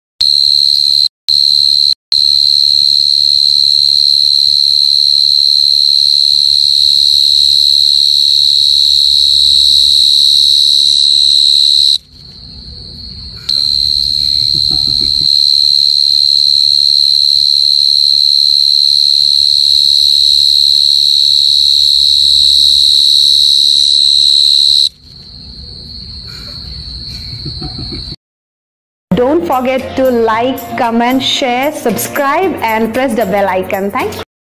Deep night cricket incept Royalty Free Sound | Cricket Insect Free Sound Affect | Ant Real Sound
Welcome to YouTube Channel *All Type Videos*

A video editing by: YouTube partner
YouTube.com/smartphonefreejob

In this video you'll see a real Sound Affect of (Cricket Insect) which you could download and use in your video music background.